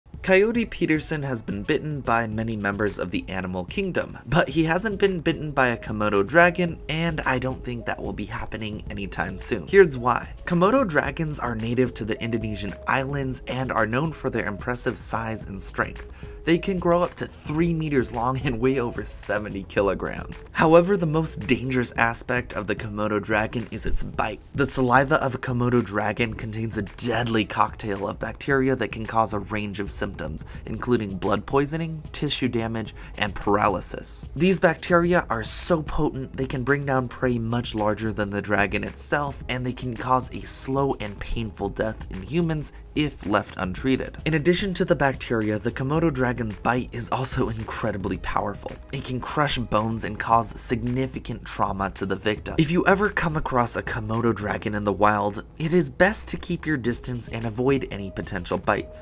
You Don't Want To Get Bit By A Komodo Dragon
In this we discuss why coyotepeterson wont be bit by a komododragon